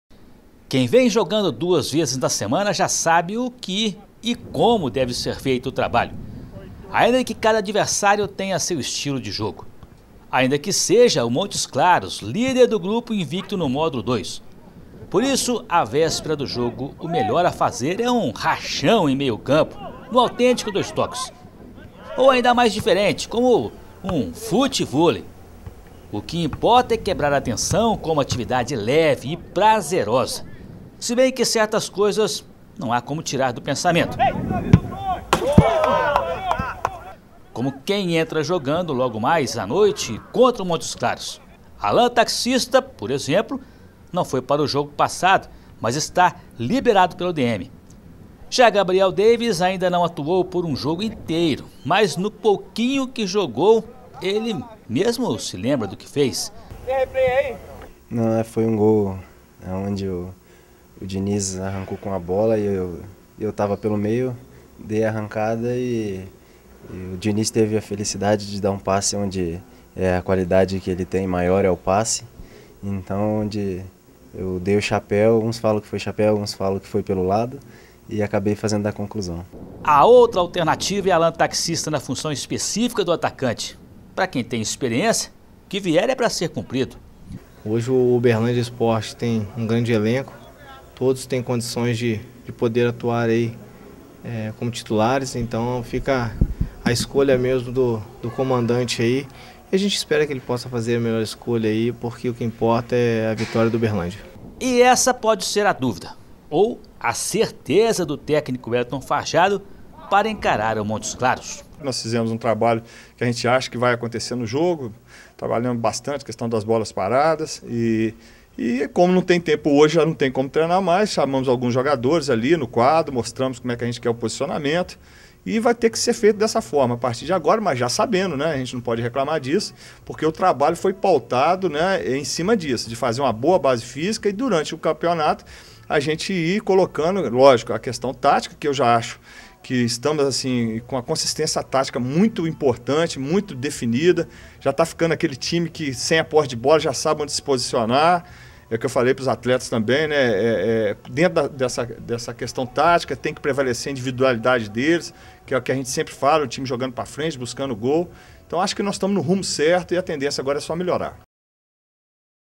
Verdão se prepara para receber o Montes Claros no Parque do Sabiá
Reportagem e imagens de Carlos Vilela para o Vitoriosa Esporte e Portal Uipi de 12/02/2014